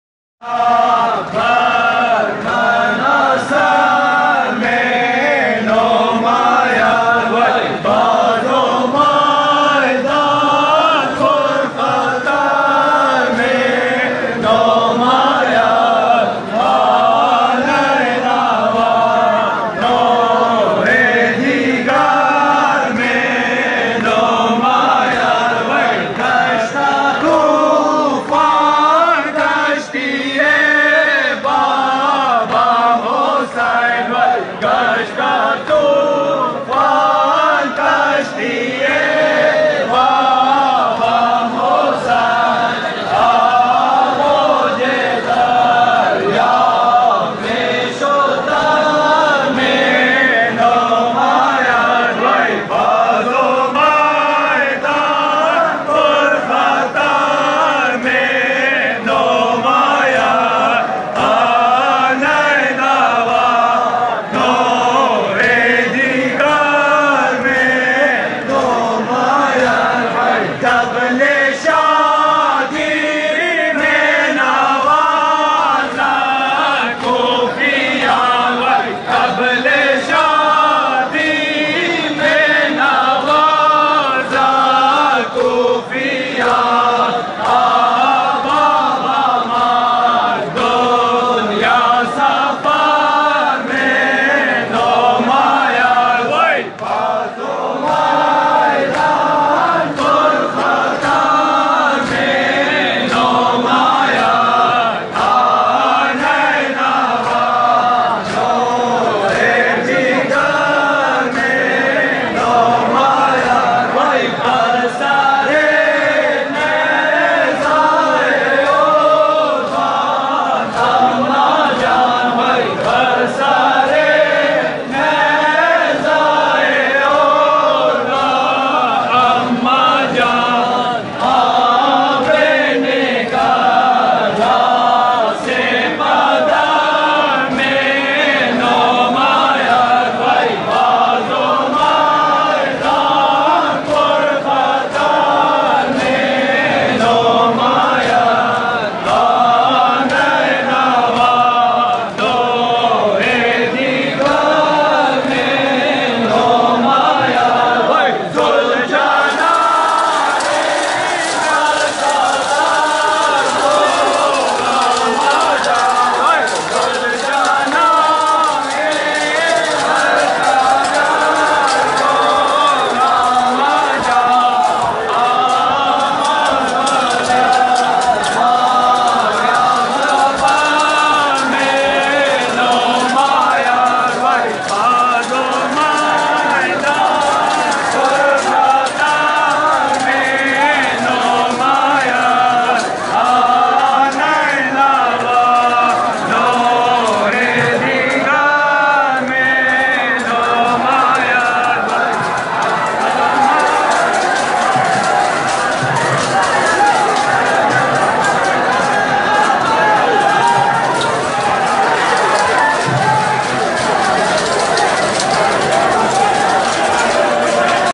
10 Muharram, Zanjeer zani Imam Bargah Haider Shah, Peshawar City.
10 Muharram : Ashora Day.
ImamBargah : Imam Bargah Haider Shah.
Location : Peshawar City.
Imam Bargah Akhundabad.